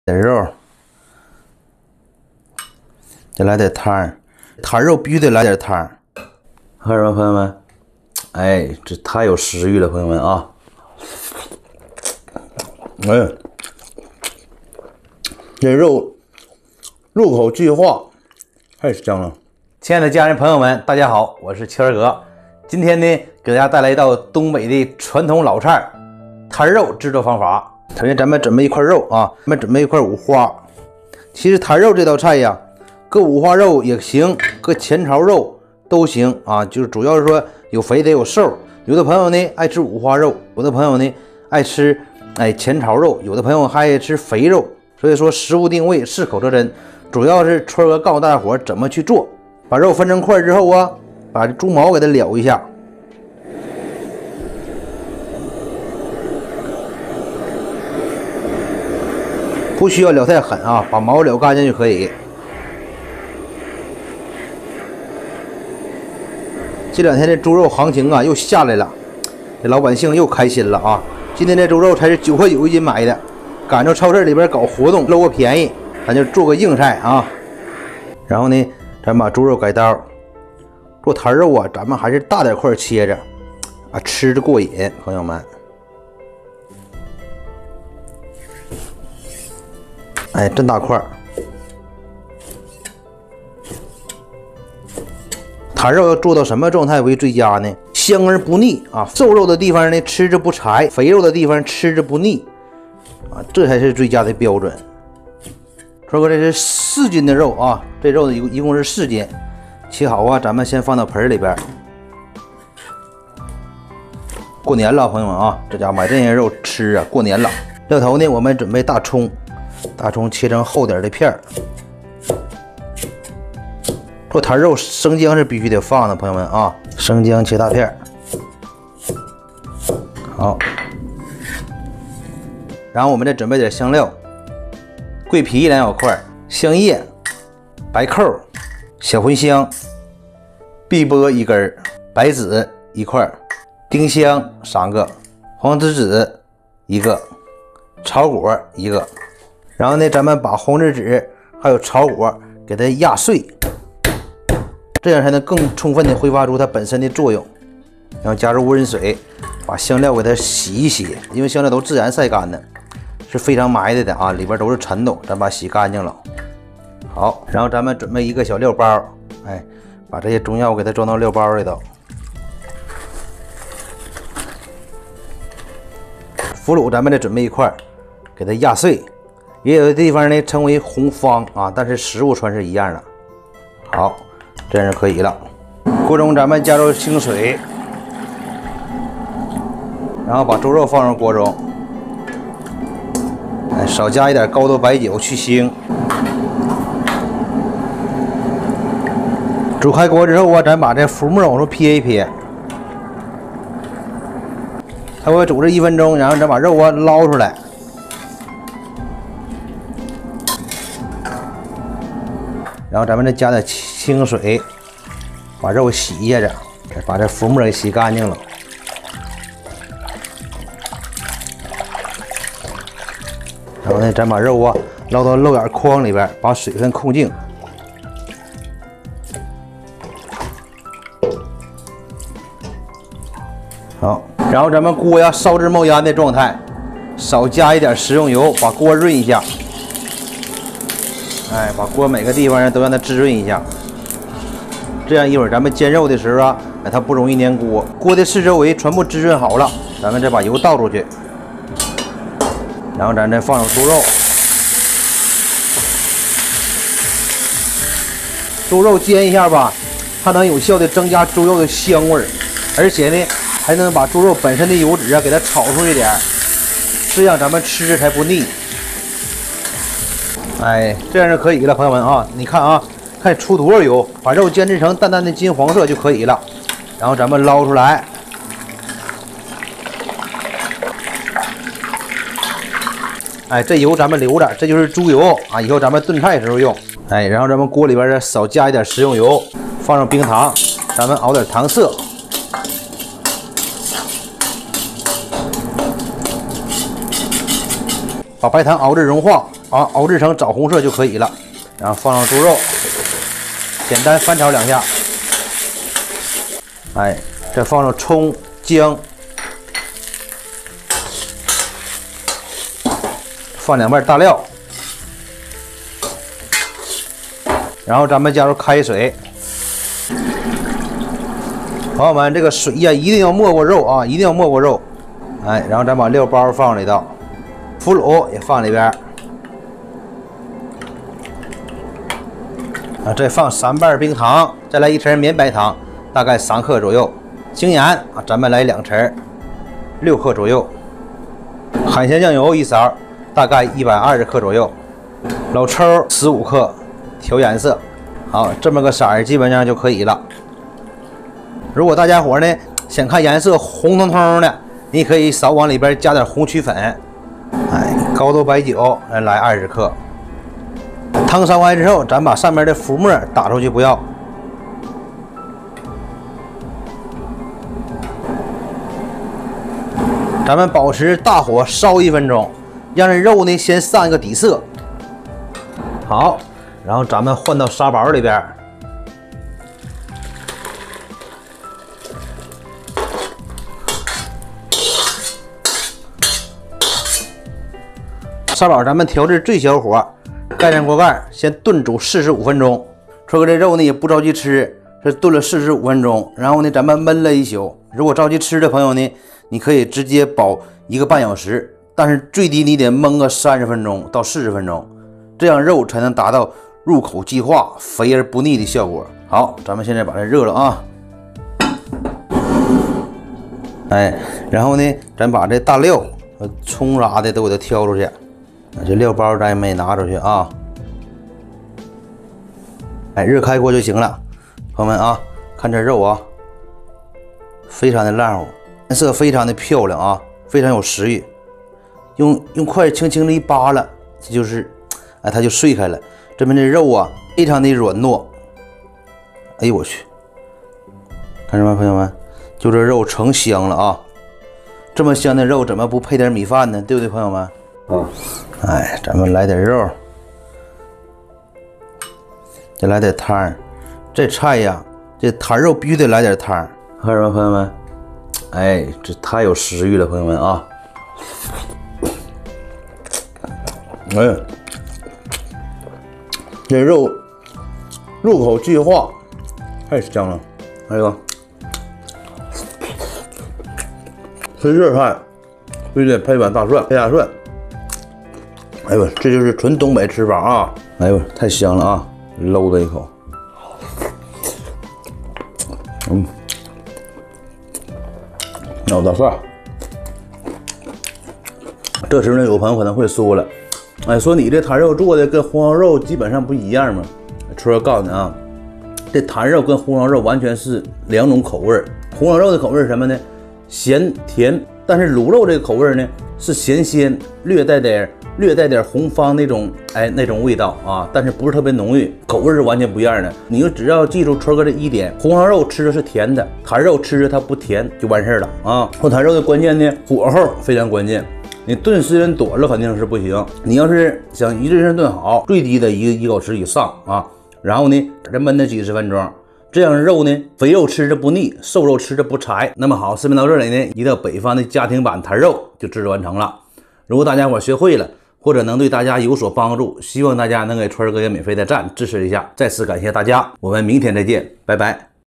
百年老店靠一道菜“坛肉”，养活一家子，猪肉肥而不腻鲜香软糯【美食圈哥】
"我们将坚持确保视频内容与动物福祉保持一致，所有食材的来源都符合法律和道德标准。" 百年老店靠一道菜“坛肉”，养活一家子，猪肉肥而不腻鲜香软糯【美食圈哥】